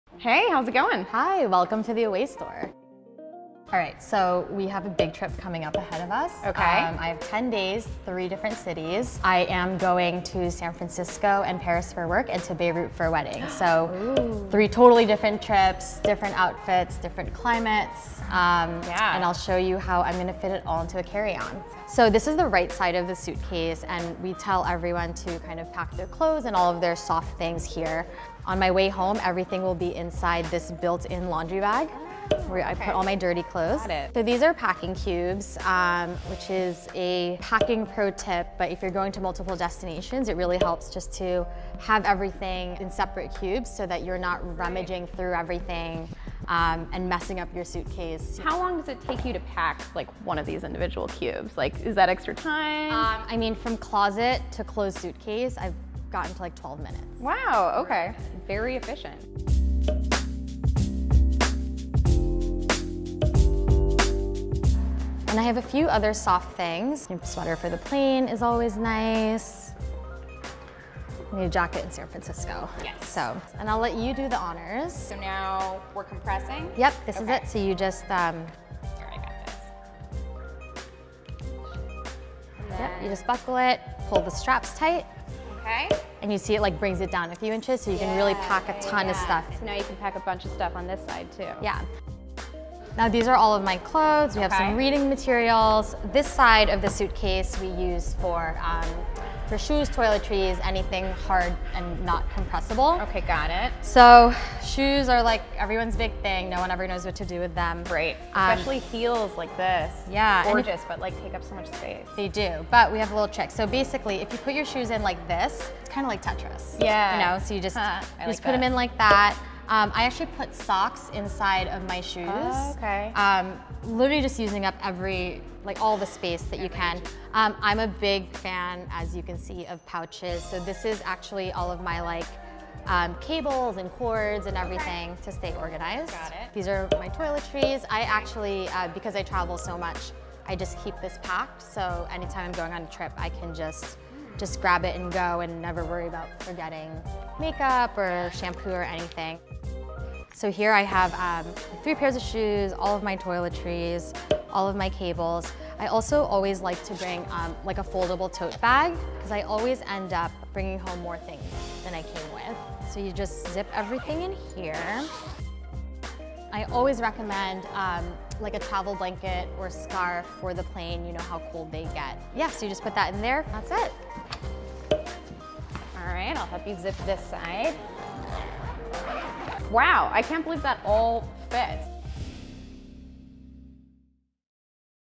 How to Pack Everything in a Carry On for a 10 Day International Trip | Inc.
Going somewhere? Away co-founder Jen Rubio reveals all her secrets for how to pack a perfect suitcase.